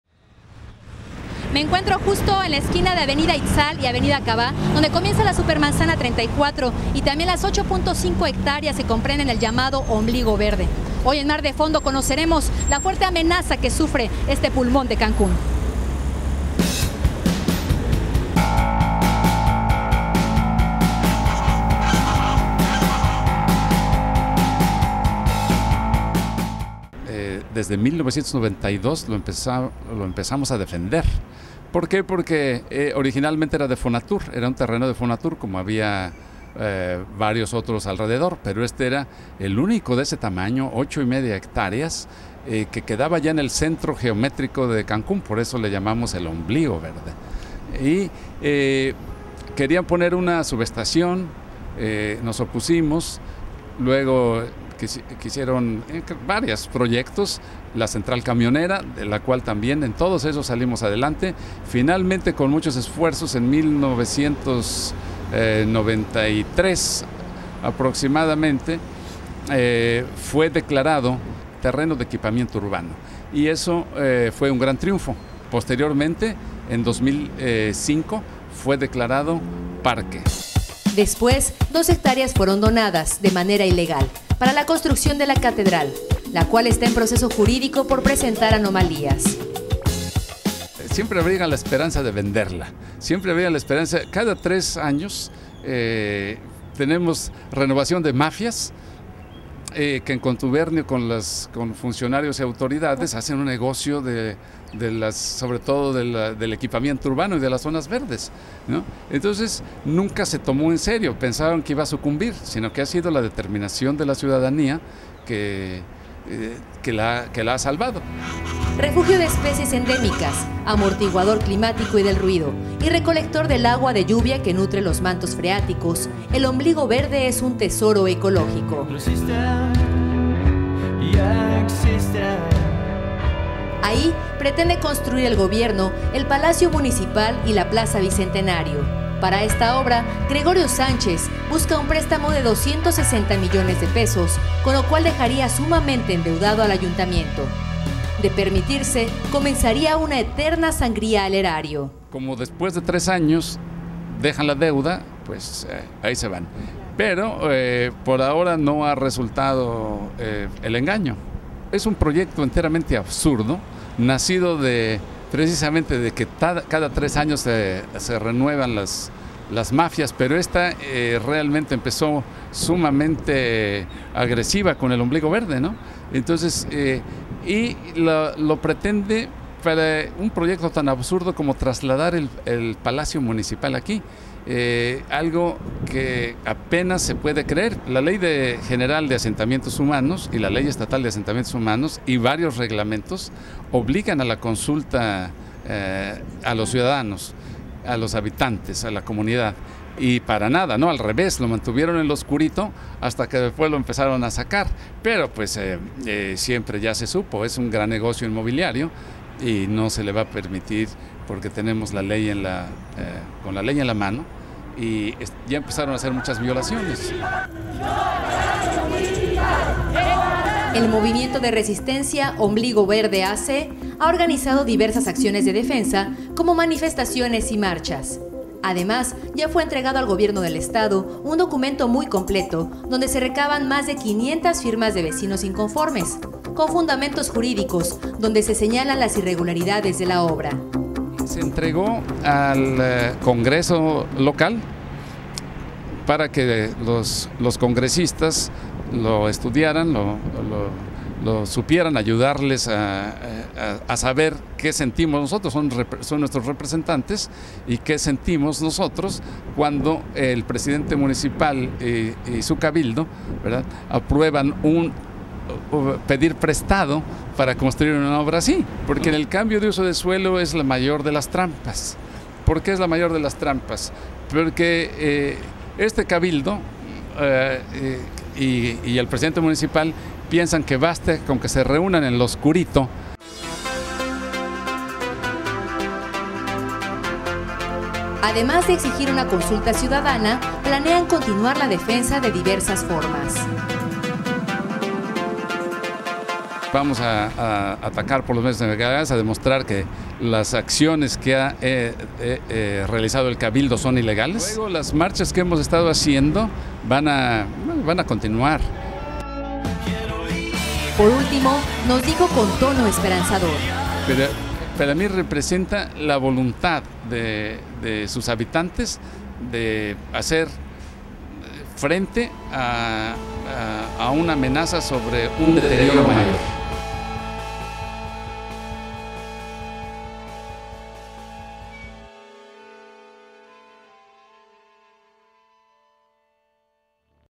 Ombligo Verde apunto de Morir...
reportaje sobre el ultimo rincón verde de lo que fue una ciudad natural, la basura del presidente municipal quiere pero no lo vamos a dejar.